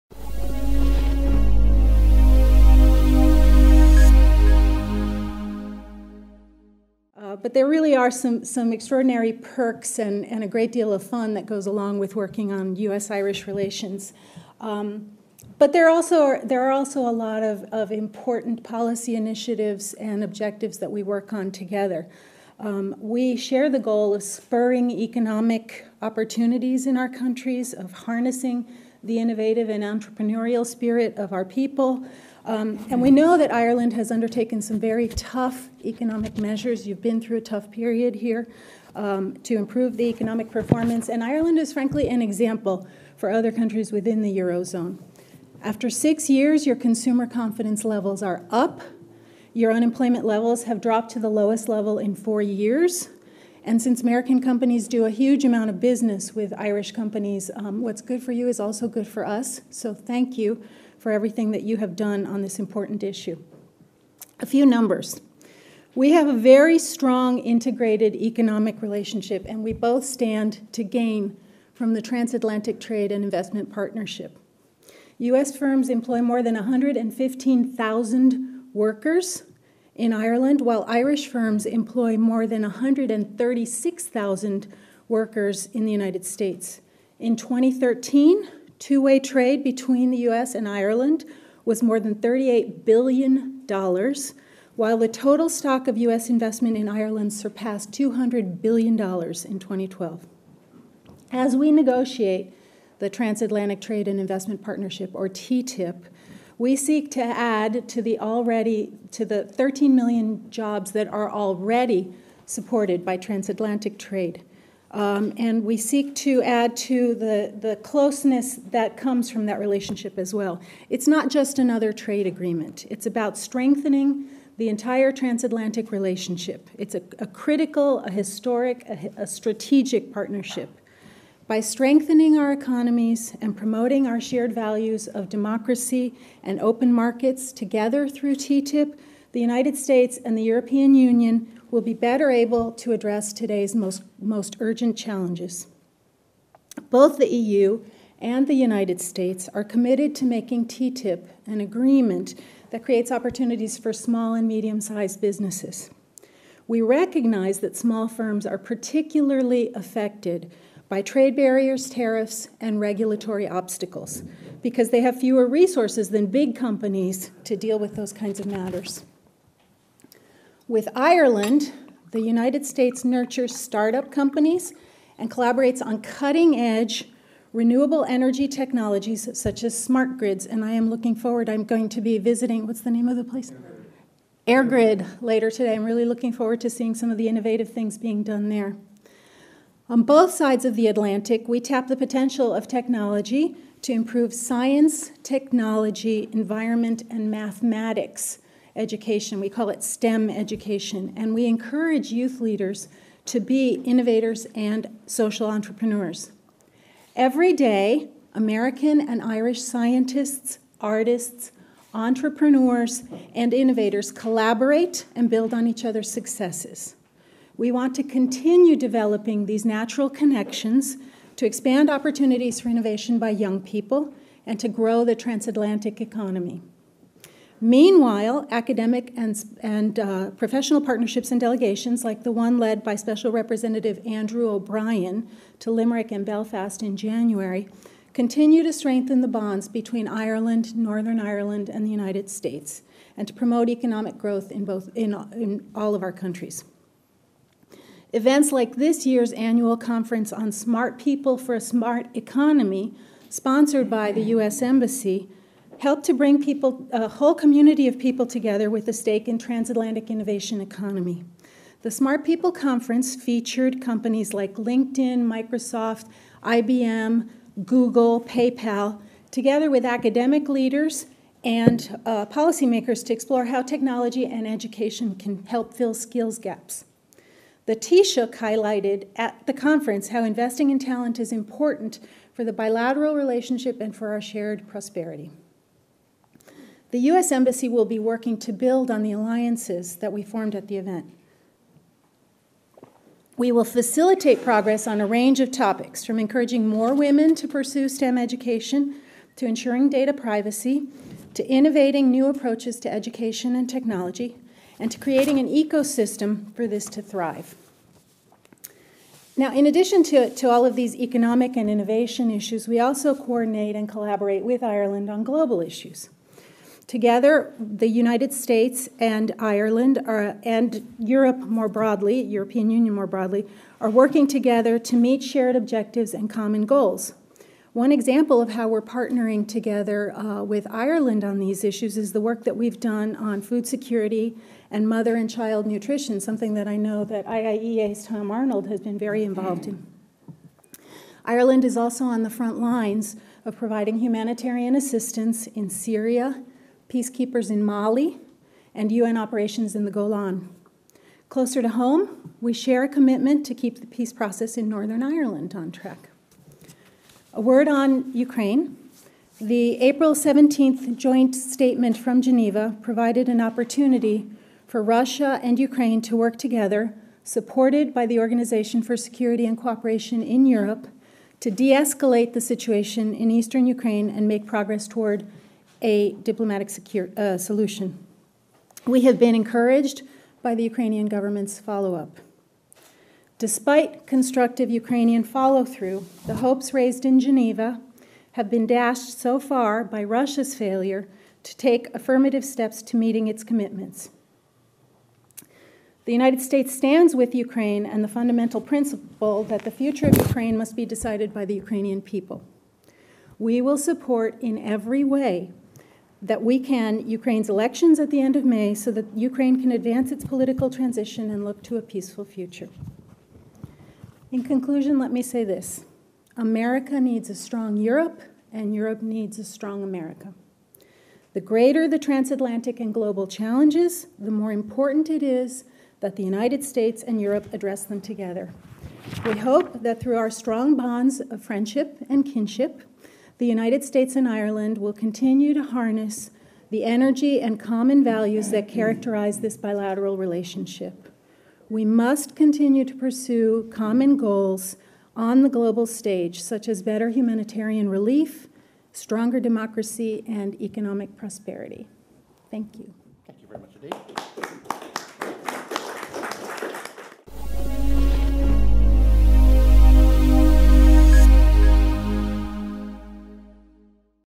Julieta Valles Noyes - U.S. Perspective on Bilateral Ties with Ireland ...
About the Speech:

After the EU-U.S. Summit in Brussels last month and in the context of a changing geopolitical environment, Julieta Valls Noyes gave an address on the future of transatlantic relations. She also provide a U.S. perspective on the Transatlantic Trade and Investment Partnership (TTIP), which is currently being negotiated by the European Union and the United States and would create the world's largest free trade area.

About the Speaker:

Julieta Valls Noyes assumed her duties as Deputy Assistant Secretary in the Bureau of European and Eurasian Affairs in July 2013. Her duties include responsibility for managing relations with the European Union and Western Europe as well as the Public Diplomacy portfolio. Previously she served as Deputy Executive Secretary for the Department of State and as Deputy Chief of Mission and Chargé d'Affaires at the U.S. Embassy to the Holy See. She has also served as the Director of the office of Multilateral and Global Affairs in the Bureau of Democracy, Human Rights and Labour.